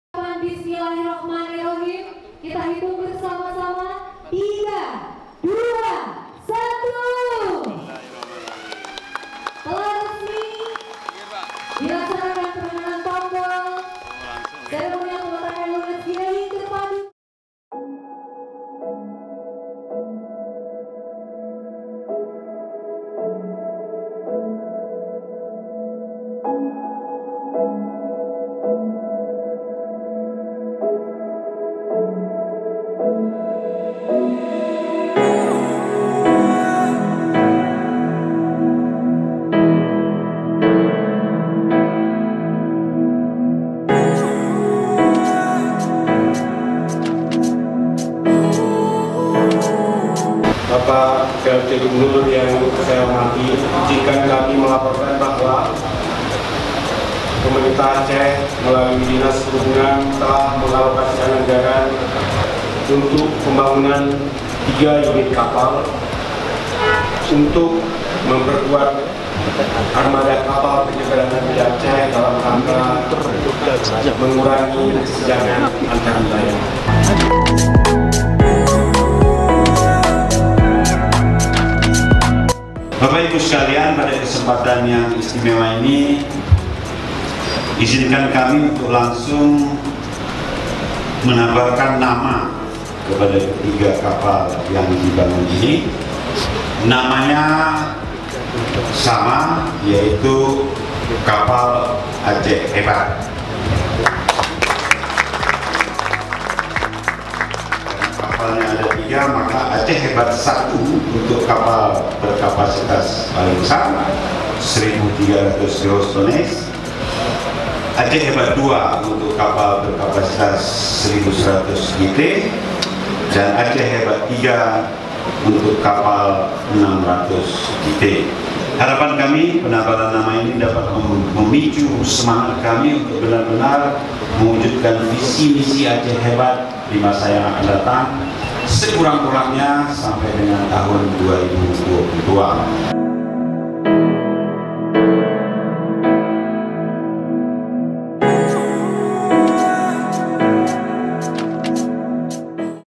KMP ACEH HEBAT, NAMA KAPAL BARU PEMERINTAH ACEH
Pelaksana Tugas Gubernur Aceh, @novairiansyah tabalkan “Aceh Hebat” sebagai nama ketiga kapal ferry Ro-Ro milik Pemerintah Aceh, Senin 21 Oktober 2019. Penabalan nama tersebut dilaksanakan saat menghadiri acara peletakan lunas (keel laying) pembangunan 3 kapal ro-ro yang dipusatkan di salah satu galangan kapal,  PT. Adiluhung Saranasegara Indonesia, Madura, Jawa Timur.

Acara peletakan lunas (keel laying) untuk ketiga kapal ro-ro pesanan Pemerintah Aceh ini dilaksanakan secara terpadu yang dipusatkan di Bangkalan, Madura. Keel laying merupakan tahapan awal dari pembangunan kapal yang dianggap sebagai hari kelahiran kapal baru.

Usai sambutan, Nova menandatangani plakat keel laying dan menabalkan nama pada ketiga kapal ro-ro sebagai tanda keel laying, yaitu; ACEH HEBAT 1 untuk kapal berkapasitas 1.300 GT; ACEH HEBAT 2 untuk kapal berkapasitas 1.100 GT; dan ACEH HEBAT 3 untuk kapal 600 GT. Nama yang diberikan ini adalah tagline dan manifestasi dari visi misi Pemerintah Aceh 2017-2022. “Harapan kami, penabalan nama ini dapat memacu semangat kami untuk benar-benar mewujudkan visi misi Aceh Hebat,” ungkap Nova dengan semangat.

Acara ini turut dihadiri Kepala Kejaksaan Tinggi Aceh, Irdam, SH, MH., Direktur Transportasi SDP, Direktorat Jenderal Perhubungan Darat, Kementerian Perhubungan RI, diwakili oleh Kasubdit Sarana Transportasi, Ir. Sri Hardianto, S.T., MM.Tr, serta para pimpinan perusahaan pelaksana pembangunan Kapal dan pengawasannya.